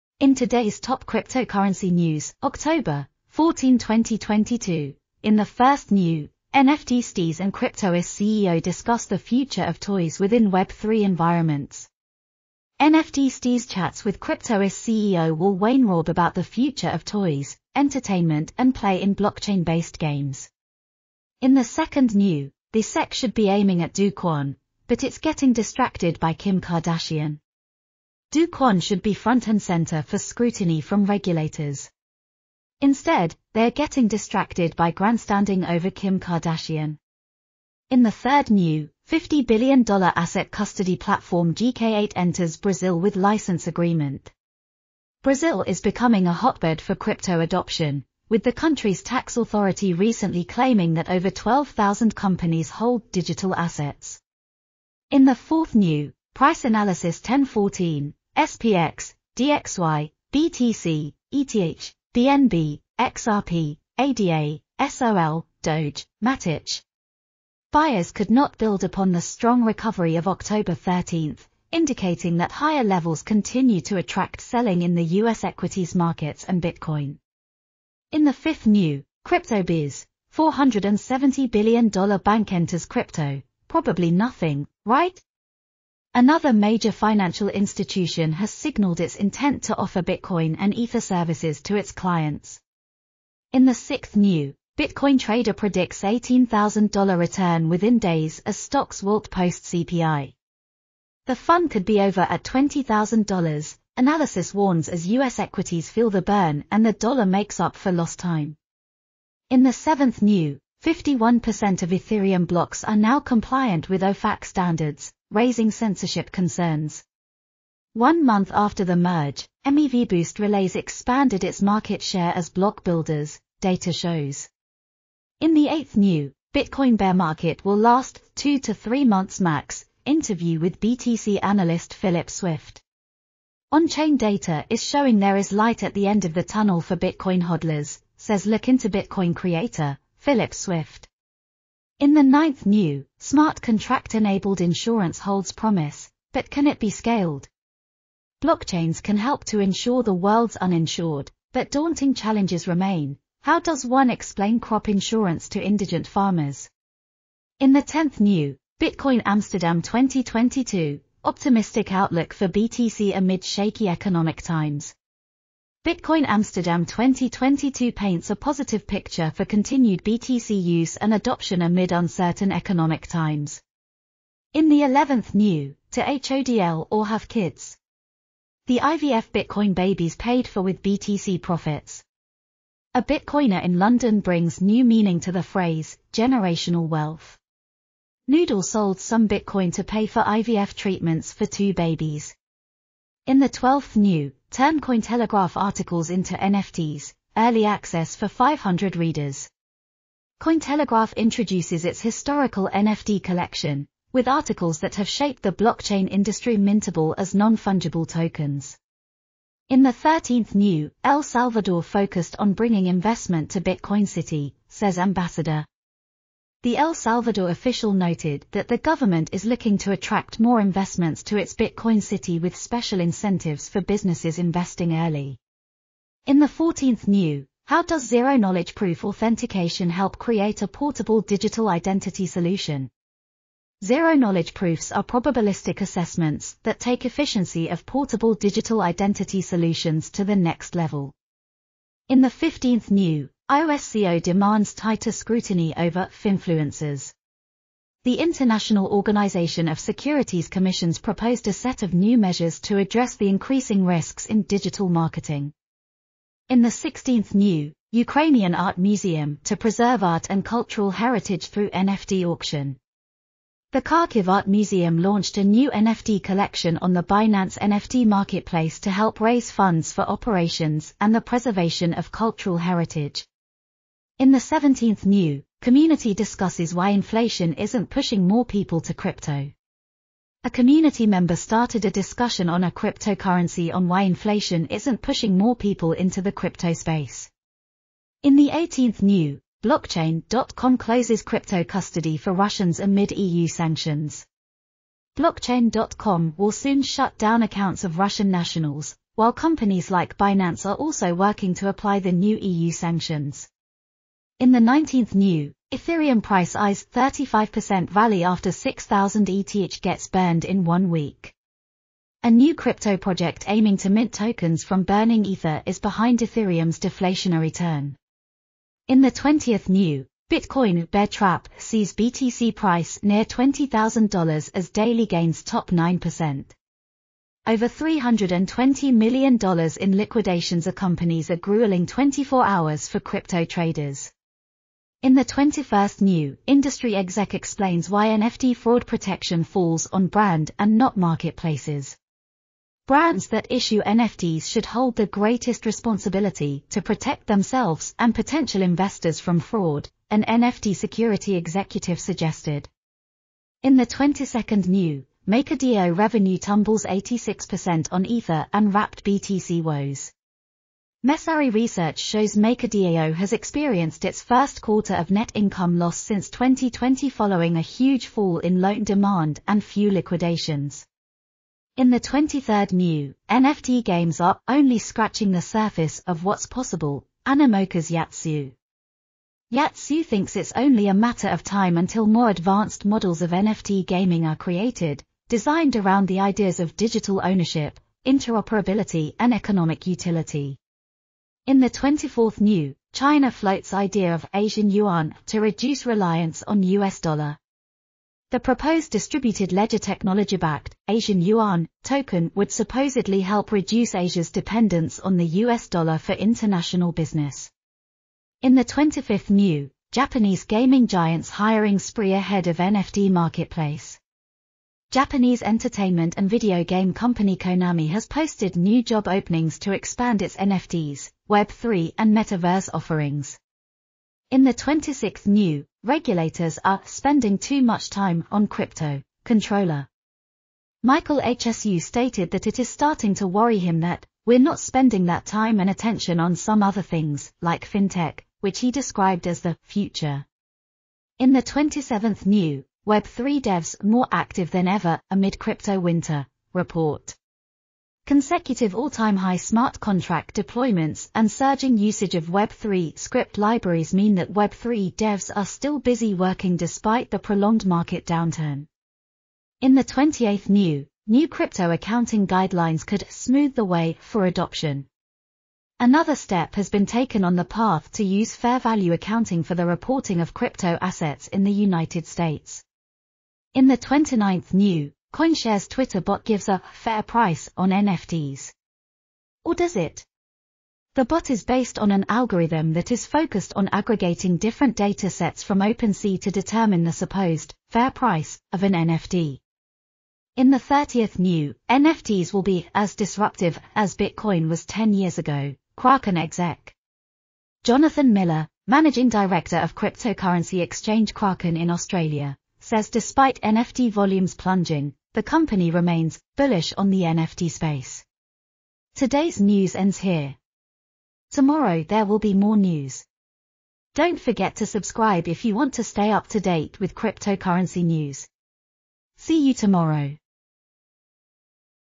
🏆 INVESTORS WARN! | Cryptocurrency NEWS | TODAY | Shiba | USD | Stellar
All the outstanding news from the world of cryptocurrencies today, day October, 14 2022.

😲  NFT Steez and Cryptoys CEO discuss the future of toys within Web3 environments
💡  The SEC should be aiming at Do Kwon — But it’s getting distracted by Kim Kardashian
💣  $50B asset custody platform GK8 enters Brazil with license agreement
⚫️  Price analysis 10 14: SPX, DXY, BTC, ETH, BNB, XRP, ADA, SOL, DOGE, MATIC
🔥  Crypto Biz: $470B bank enters crypto — Probably nothing, right?
😲  Bitcoin trader predicts $18K return within days as stocks wilt post-CPI
💡  51% of Ethereum blocks are now compliant with OFAC standards, raising censorship concerns
➡️  Bitcoin bear market will last 2-3 months max — Interview with BTC analyst Philip Swift
💣  Smart contract-enabled insurance holds promise, but can it be scaled?
🔥  Bitcoin Amsterdam 2022: Optimistic outlook for BTC amid shaky economic times
🔴  To HODL or have kids? The IVF Bitcoin Babies paid for with BTC profits
🏆  Turn Cointelegraph articles into NFTs — Early access for 500 readers
🌟  El Salvador focused on bringing investment to Bitcoin City, says ambassador
🤯  How does zero-knowledge proof authentication help create a portable digital identity solution?
➡️  IOSCO demands tighter scrutiny over finfluencers
👉🏼  Ukrainian art museum to preserve art and cultural heritage through NFT auction
▶️  Community discusses why inflation isnt pushing more people to crypto
✔️  Blockchain​.com closes crypto custody for Russians amid EU sanctions
🔴  Ethereum price eyes 35% rally after 6,000 ETH gets burned in one week
🟢  Bitcoin ‘bear trap’ sees BTC price near $20K as daily gains top 9%
💣  Industry exec explains why NFT fraud protection falls on brand and not marketplaces
🟢  MakerDAO revenue tumbles 86% on Ether and Wrapped BTC woes
🌟  NFT games are ‘only scratching the surface’ of whats possible — Animoca’s Yat Siu
😱  China floats idea of ‘Asian yuan’ to reduce reliance on US dollar
🥇  Japanese gaming giant’s hiring spree ahead of NFT marketplace
⚫️  Regulators are ‘spending too much time’ on crypto: Comptroller
🤯  Web3 devs ‘more active than ever’ amid crypto winter: Report​​
✅  New crypto accounting guidelines could ‘smooth the way’ for adoption
⭐️  CoinShares’ Twitter bot gives a ‘fair price’ on NFTs... or does it?
⚫️  NFTs will be ‘as disruptive’ as Bitcoin was 10 years ago — Kraken exec

We hope you liked todays video and crypto news!